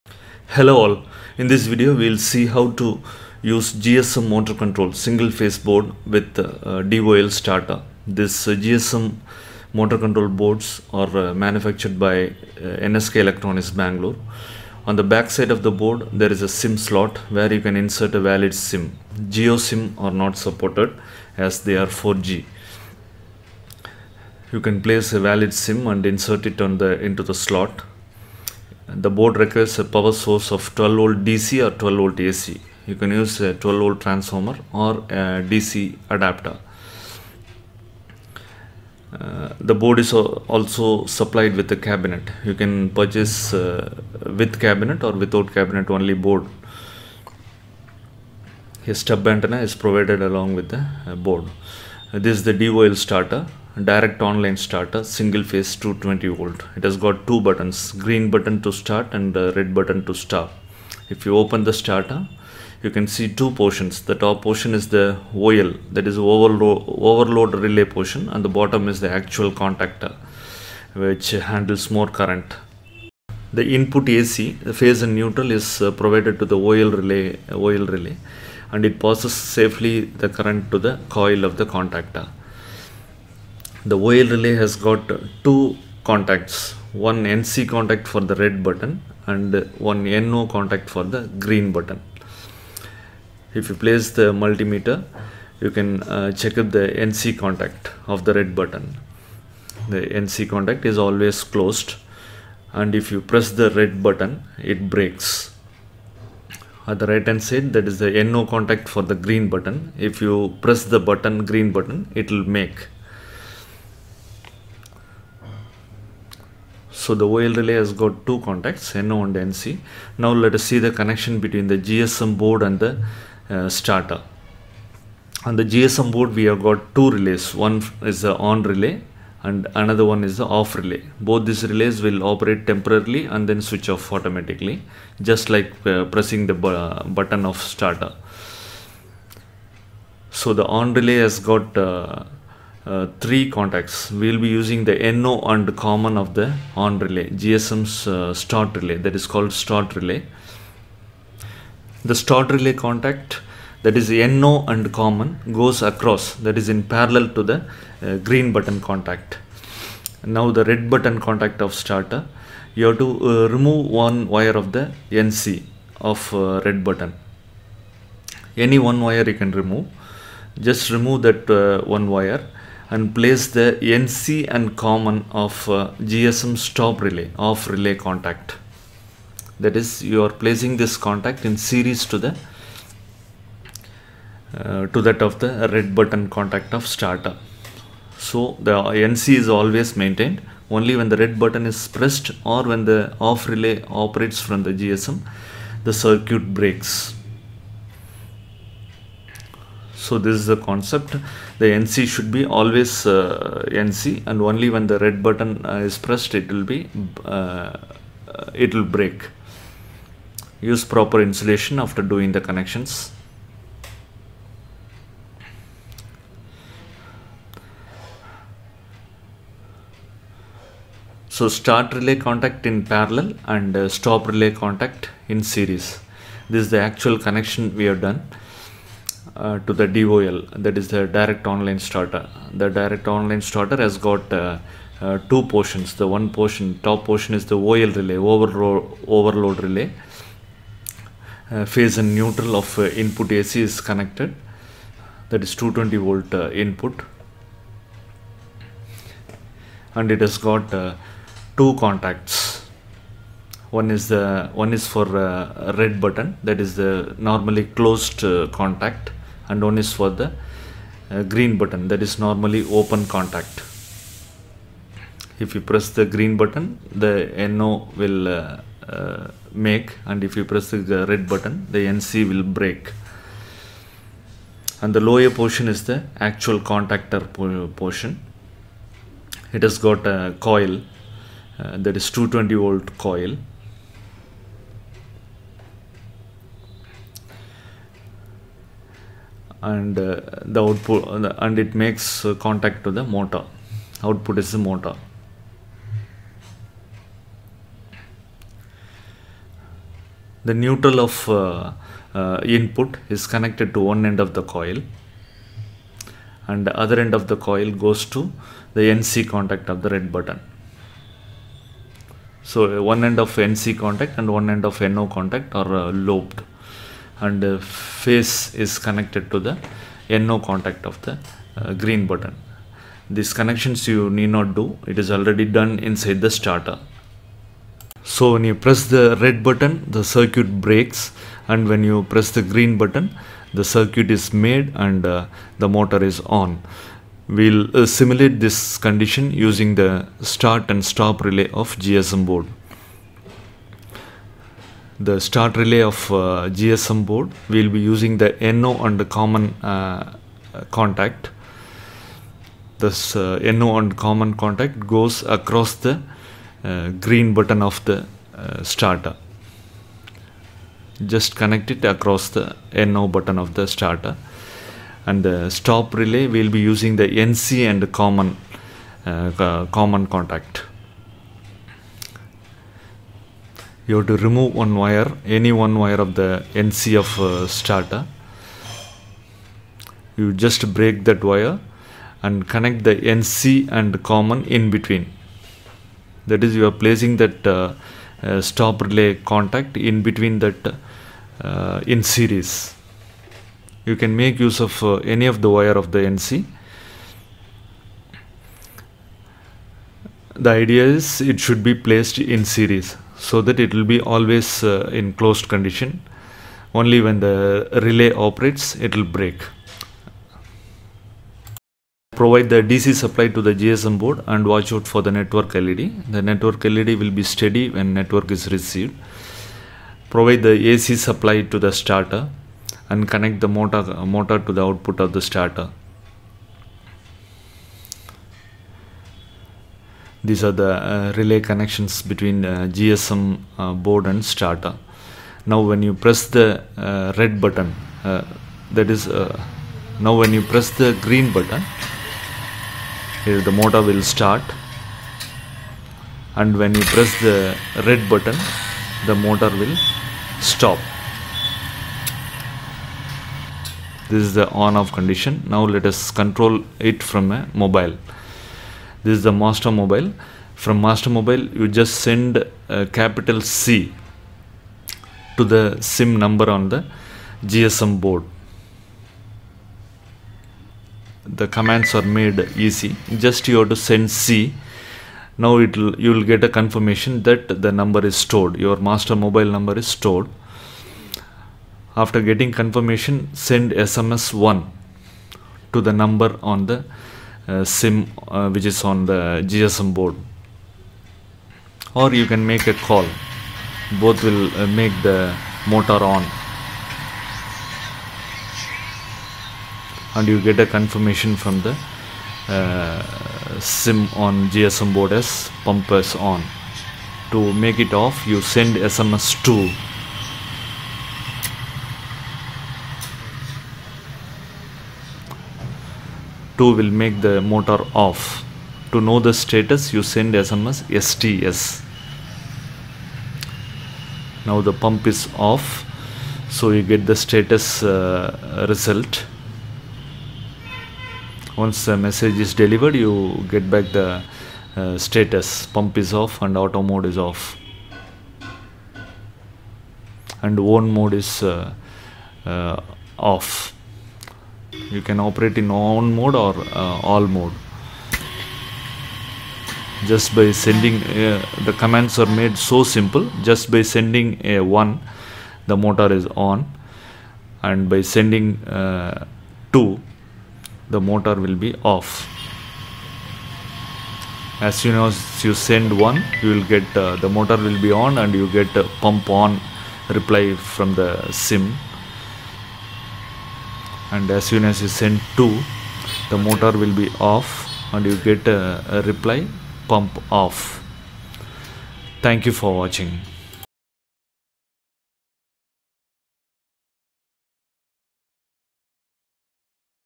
GSM MOTOR CONTROL STARTER WIRING DEMO SINGLE PHASE
GSM based Motor control board is convenient t control from home ,Motor in Farm lands.
DOL Direct On Line Starter is used with single phase Motors. This video is on how to do the connections between GSM board and the STARTER.